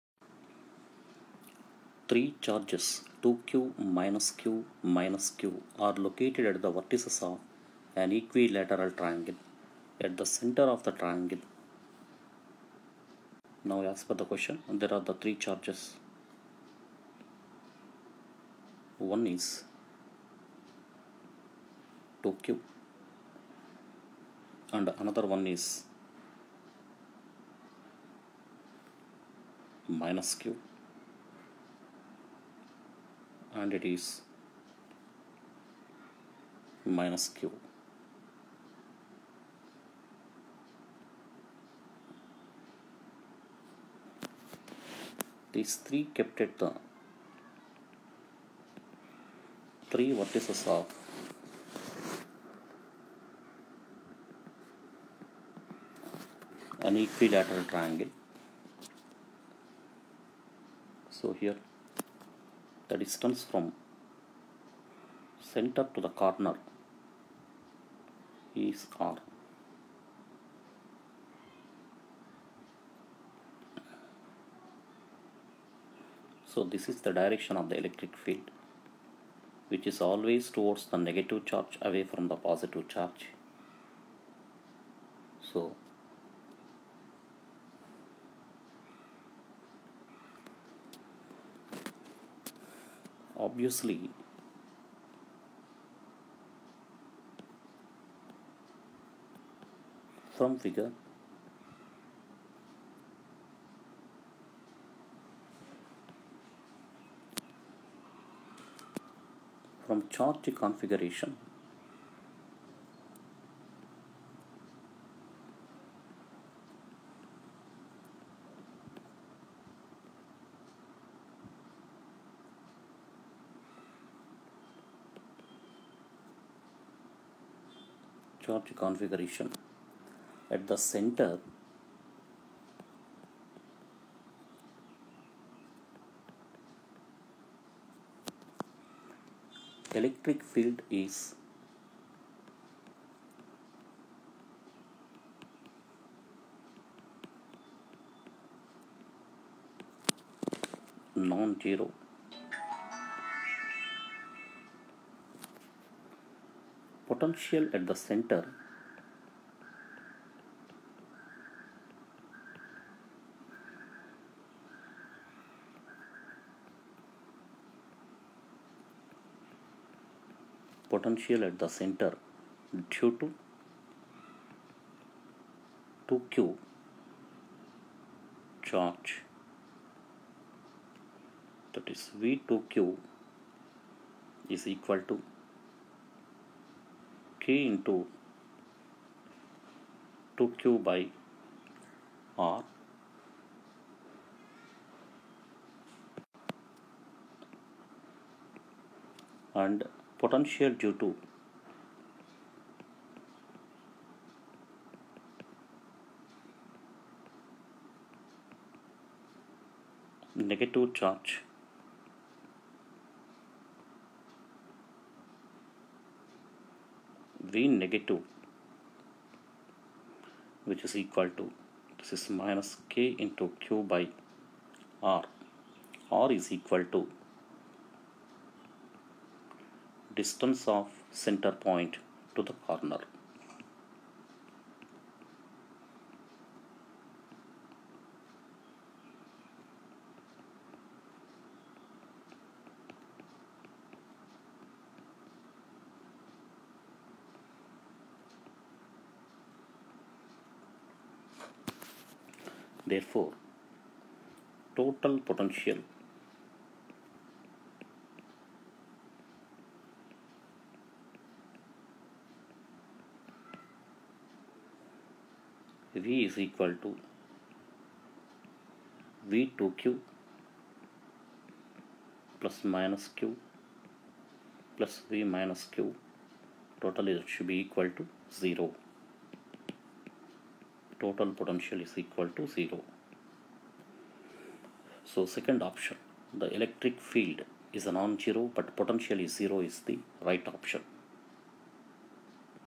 Electric Charges & Fields - Electric Field Due to Point Charges & Continuous Charge
Three charges 2q, -q, -q are located at the vertices of an equilateral triangle. At the centre of the triangle: